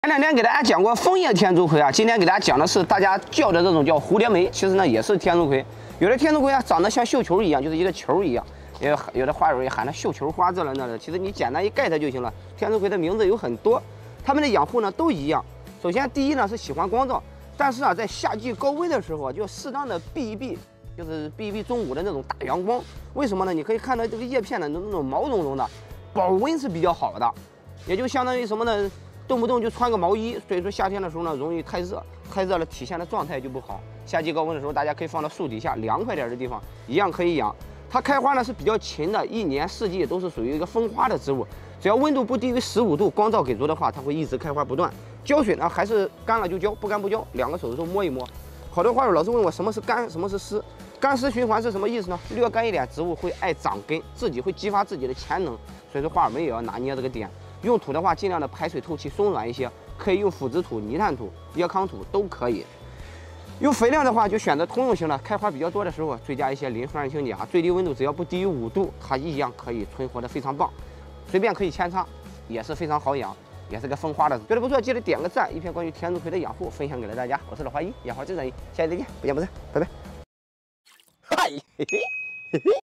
【养花必懂小知识】只见花海不见叶，名副其实的“爆盆神器”！蝴蝶梅养护教学！ Tips and tracks for Geranium.
天竺葵？蝴蝶梅？绣球花？傻傻分不清？其实它们是一种花！
Tips and tracks for Calla Lily.